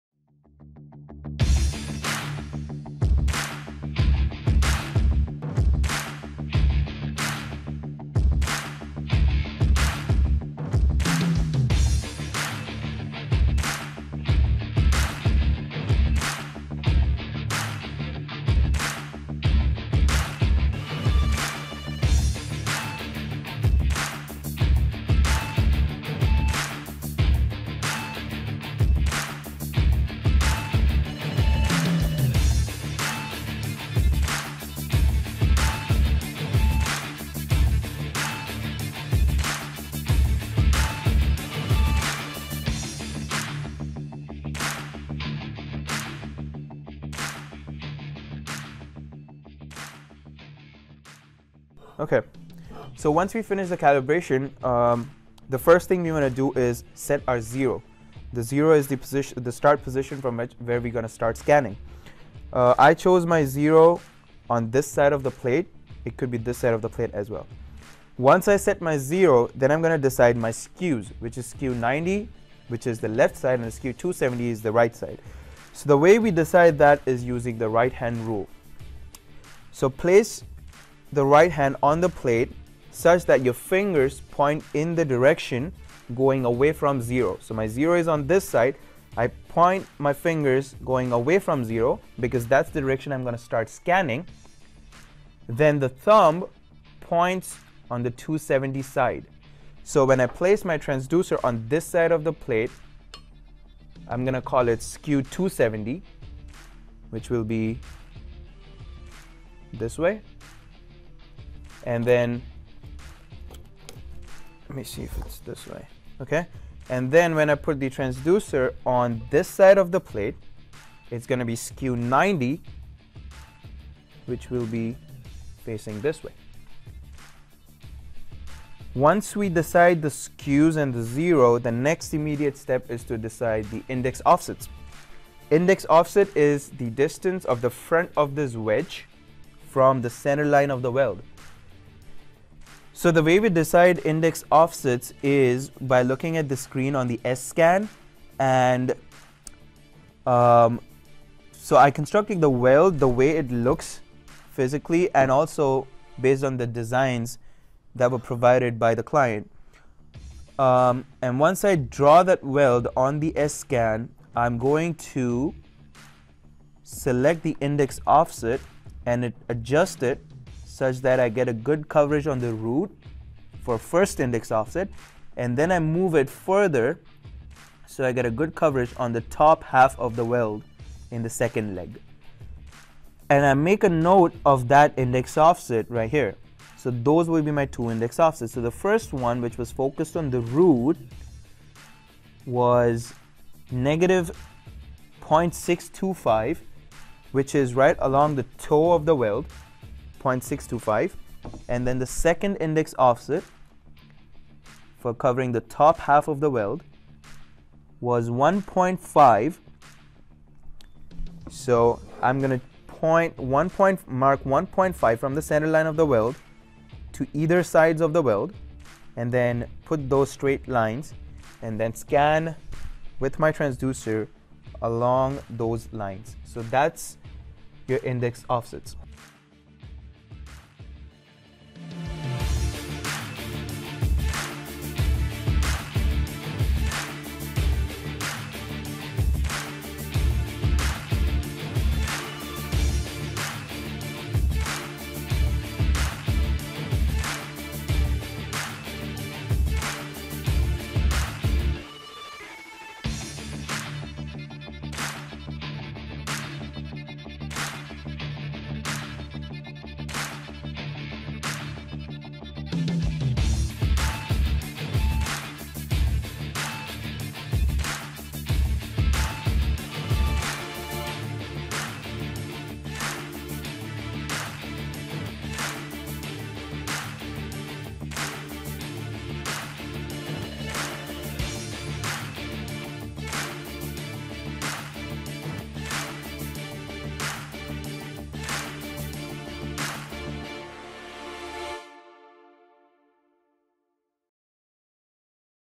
What are Skews and Index Offsets in PAUT?
This video talks about skews and index offsets, how to decide on your values for those and some suggestions like right hand rule to stay consistent with your skew choice. Let me know what you think. I appreciate criticism too.